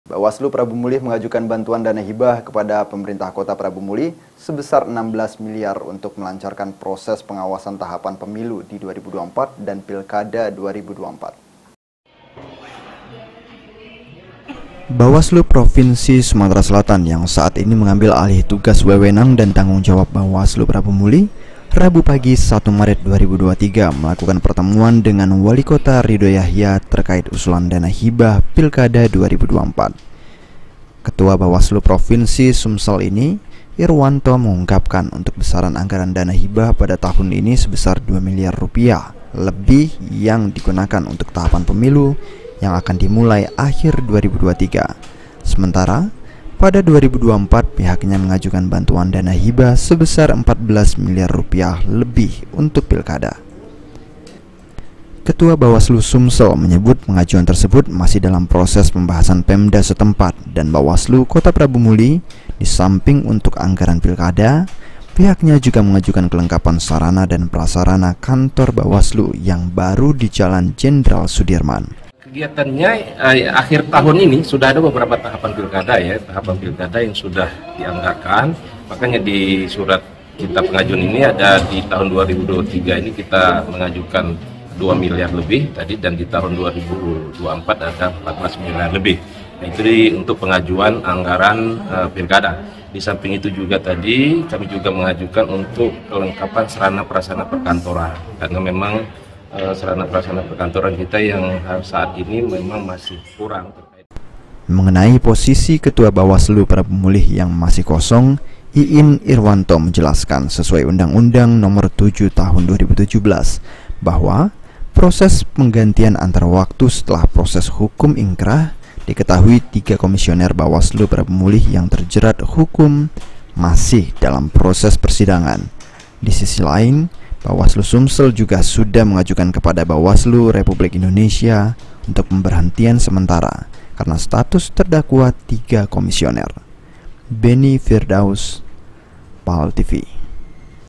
Bawaslu Prabumulih Ajukan Dana Hibah 16 Miliar | Paltv
Bawaslu Prabumulih Mengajukan Bantuan Dana Hibah Kepada Pemko Prabumulih Sebesar 16 Miliar Untuk Melancarkan Proses Pengawasan Tahapan Pemilu Di 2024 Dan Pilkada 2024 ///